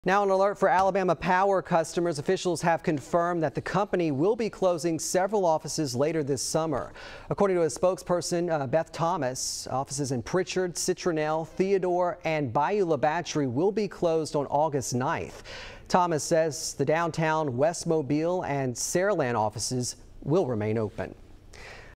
Alabama Power to close offices in Prichard, Citronelle, Theodore and Bayou la Batre
According to spokesperson Beth Thomas, offices in Prichard, Citronelle, Theodore and Bayou la Batre are included in these closures which will take place on August 9. Thomas says the downtown, West Mobile and Saraland offices will remain open.